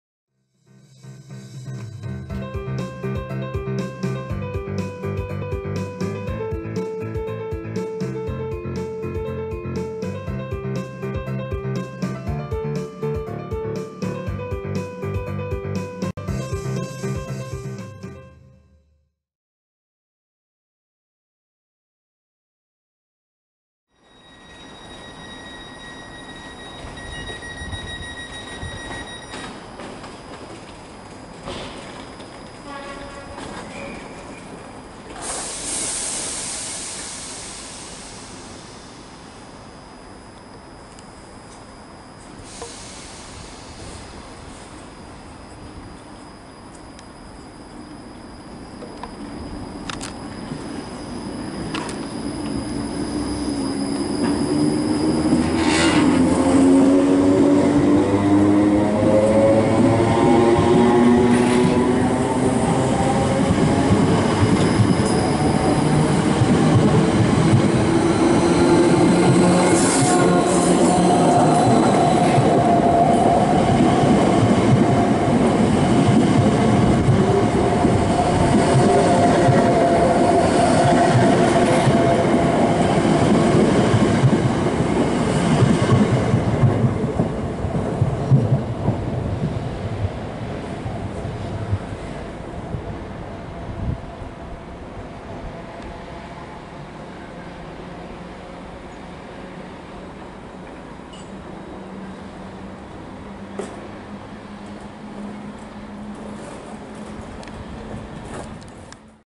321302 321311 and 321437 departing Ipswich heading for London 29/11/17
Greater Anglia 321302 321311 and 321437 departing Ipswich heading for London